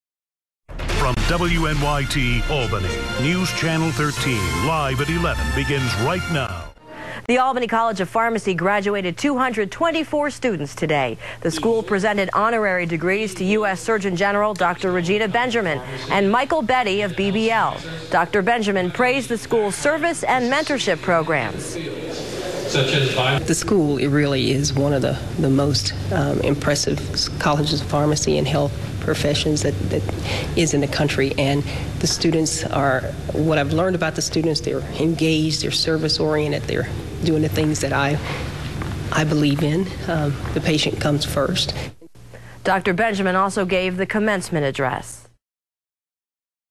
Surgeon General Regina Benjamin Delivers ACPHS Commencement Address
U.S. Surgeon General Regina M. Benjamin talks with television reporters prior to giving the commencement address at Albany College of Pharmacy and Health Sciences' 2010 graduation.